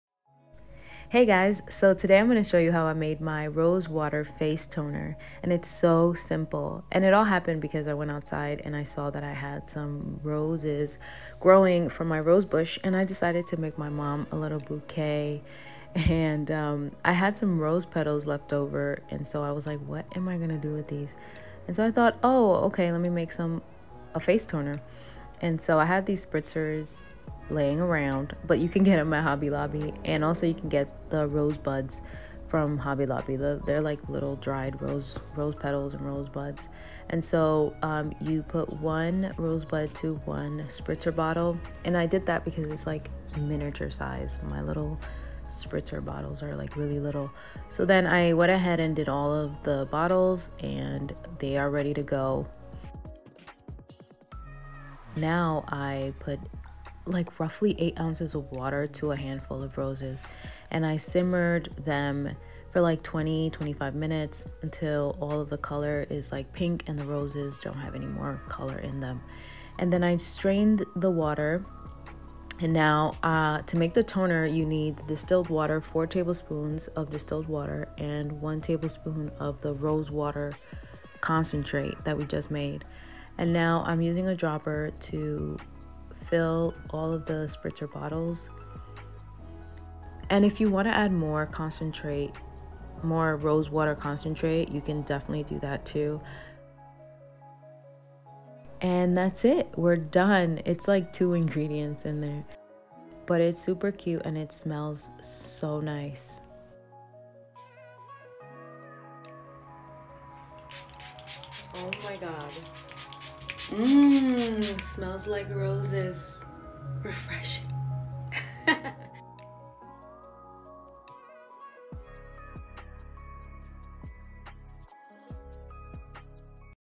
Rose Water Face Toner | Easy DIY Tutorial | All Natural Skincare
In this video I show you how to make rose water and rose water face toner. So easy you only need 2 ingredients!

1. Cleaned fresh roses
2. Filtered Water

I hope you enjoy and let me know if you made some for yourself :)

The satin bonnet that I'm wearing is made by me go visit my Etsy shop for cute accessories!

Musician: @iksonmusic